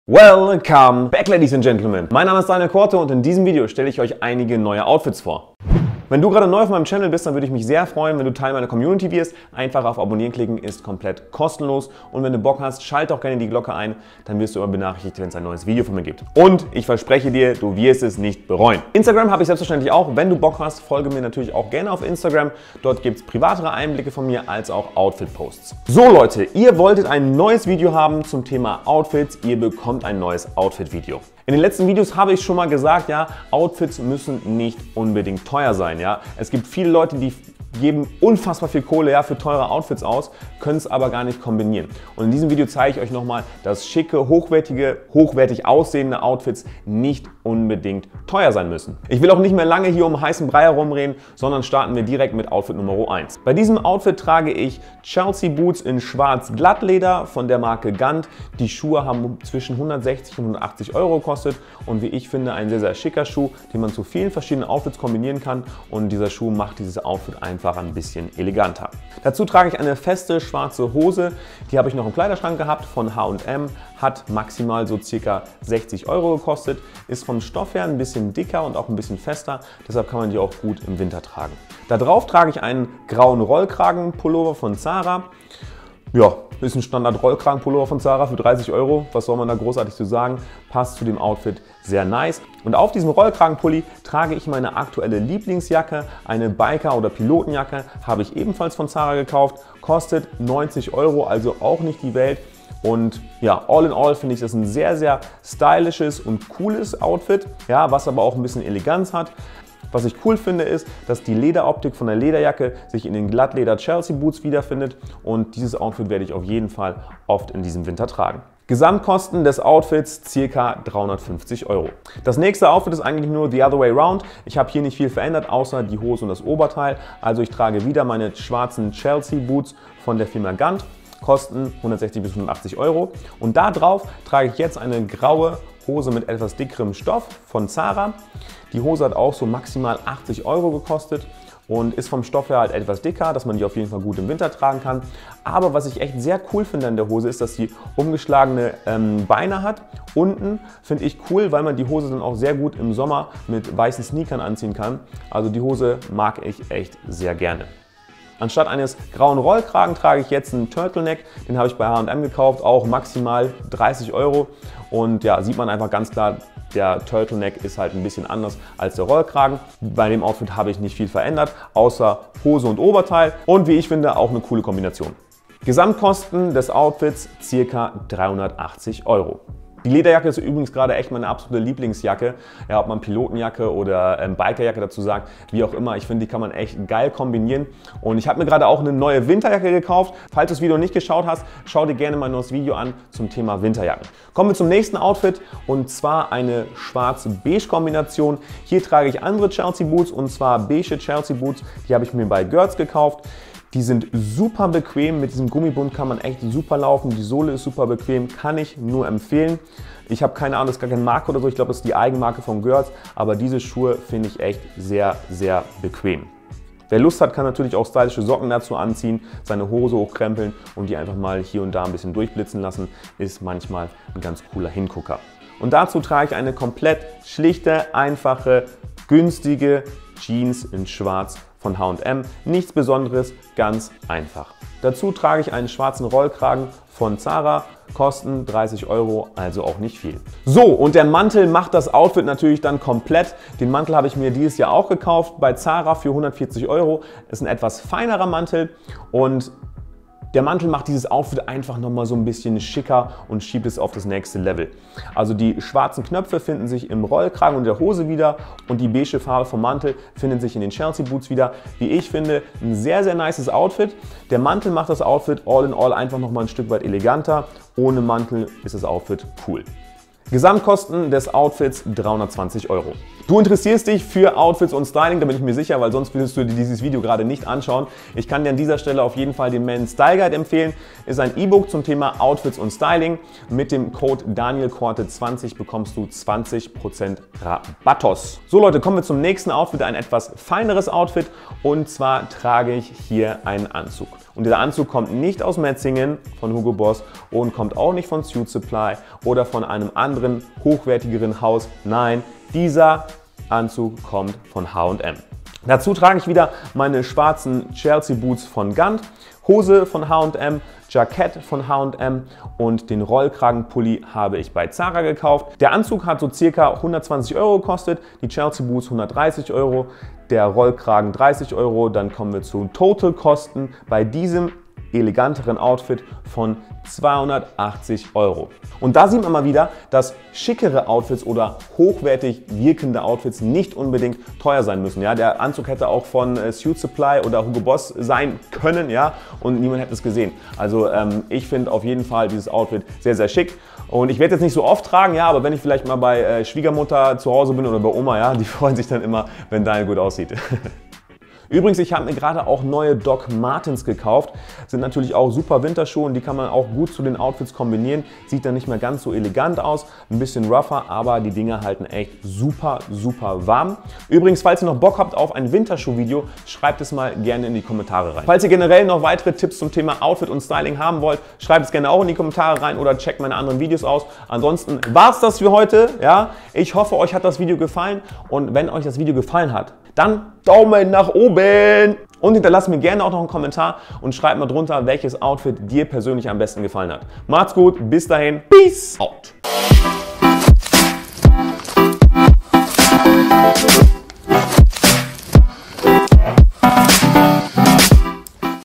STYLING TIPPS ● MÄNNER OUTFITS WINTER 2018 | DANIEL KORTE
Hey Leute,
heute zeige ich mal wieder einige Männer Outfits die ich im Winter jetzt tragen werden. Wenn ihr mehr Styling Tipps für Männer haben wollt, schreibt es gerne in die Kommentare. Nachfolgend auch einige Teile der Outfits, die ich auf Amazon gefunden habe und die denen ähneln, die ich im Video trage.

M E N S T Y L E    G U I D E